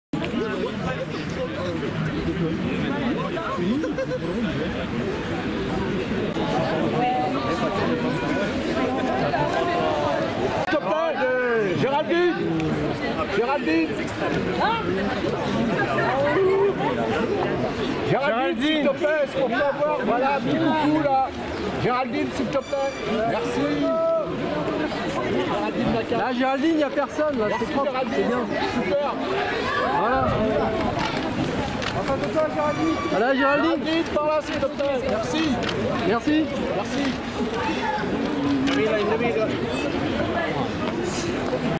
Géraldine Nakache - Défilé Paco Rabanne Automne/Hiver 2023/2024 à Paris - 01.03.2023
L'actrice et réalisatrice Géraldine Nakache présente au défilé Paco Rabanne Automne/Hiver 2023/2024 à Paris, le 1er Mars 2023.